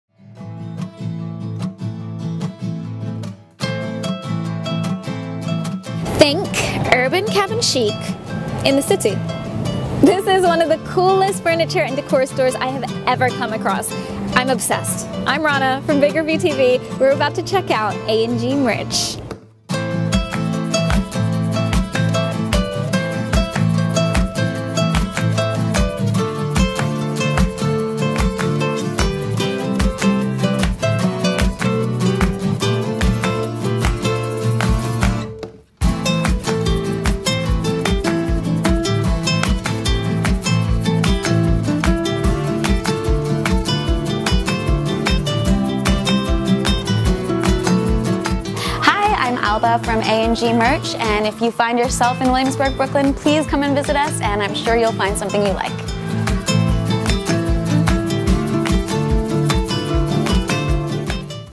A&G Merch a Home Goods Store in New York offering Homeware or Homegoods
A&G Merch strives to bring an urban, youthful energy to all our products. They have a collection of utilitarian objects ranging from couches to candlesticks, each with a uniquely urban personality.

A&G Merch
111 N 6th Street Brooklyn NY USA 1121